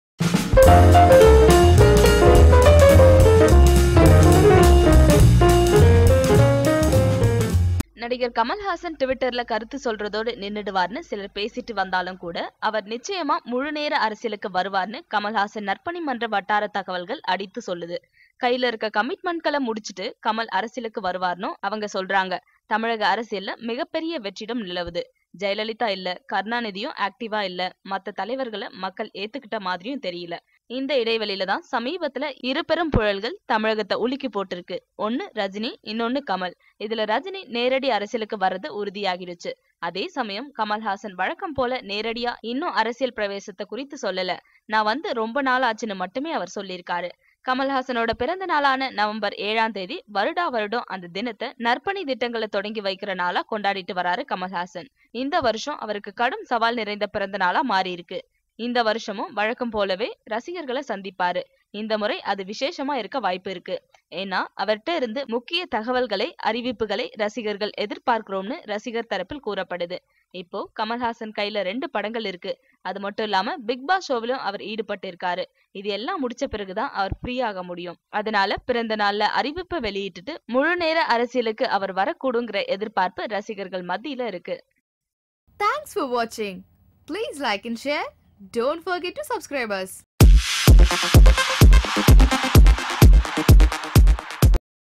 கையில் உள்ள படங்களை முடித்துவிட்டு நிச்சயம் அரசியலுக்கு வருகிறார் கமல்...!
நடிகர் கமல்ஹாசன் டிவிட்டரில் கருத்துக்கள் கூறுவதோடு நின்று விடுவார் என்று சிலர் பேசி வந்தாலும் கூட அவர் நிச்சயம் முழு நேர அரசியலுக்கு வருவார் என்று கமல்ஹாசன் நற்பணி மன்ற வட்டாரத் தகவல்கள் அடித்துக் கூறுகின்றன. கையில் உள்ள கமிட்மெண்ட்களை முடித்து விட்டு கமல் அரசியலுக்கு வருவார் என்றும் அவர்கள் கூறுகின்றனர்.

தமிழக அரசியலில் மிகப் பெரிய வெற்றிடம் நிலவுகிறது. ஜெயலலிதா இல்லை, கருணாநிதியும் ஆக்டிவாக இல்லை. மற்ற தலைவர்களை மக்கள் ஏற்றுக் கொண்டது போலவும் தெரியவில்லை.

இந்த இடைவெளியில்தான் சமீபத்தில் இரு பெரும் புயல்கள் தமிழகத்தை உலுக்கிப் போட்டுள்ளன. ஒன்று ரஜினி, இன்னொன்று கமல். இதில் ரஜினி நேரடி அரசியலுக்கு வருவது உறுதியாகி விட்டது. அதேசமயம், கமல்ஹாசன் வழக்கம் போல நேரடியாக இன்னும் அரசியல் பிரவேசம் குறித்து சொல்லவில்லை. நான் வந்து ரொம்ப நாளாச்சு என்று மட்டுமே அவர் கூறியுள்ளார்.

கமல்ஹாசனின் பிறந்த நாள் நவம்பர் 7ம் தேதி. வருடா வருடம் அந்த, தினத்தை நற்பணித் திட்டங்களை தொடங்கி வைக்கும் நாளாக கொண்டாடி வருகிறார். இந்த வருடம் அவருக்கு கடும் சவால் நிறைந்த பிறந்த நாளாக மாறியுள்ளது.

இந்த வருடமும் வழக்கம் போல ரசிகர்களை சந்திப்பார். இந்த முறை அது விசேஷமாக இருக்க வாய்ப்புள்ளது. காரணம், அவரிடமிருந்து முக்கிய தகவல்களை, அறிவிப்புகளை ரசிகர்கள் எதிர்பார்க்கிறோம் என்று ரசிகர்கள் தரப்பில் கூறப்படுகிறது.

தற்போது கமல்ஹாசன் கையில் இரு படங்கள் உள்ளன. மேலும் பிக் பாஸ் ஷோவிலும் ஈடுபட்டுள்ளார். இதையெல்லாம் முடித்த பிறகுதான் அவர் ஃப்ரீயாக முடியும். எனவே பிறந்த நாளில் அறிவிப்பை வெளியிட்டு விட்டு முழு நேர அரசியலுக்கு அவர் வரக் கூடும் என்ற எதிர்பார்ப்பு ரசிகர்கள் மத்தியில் உள்ளது.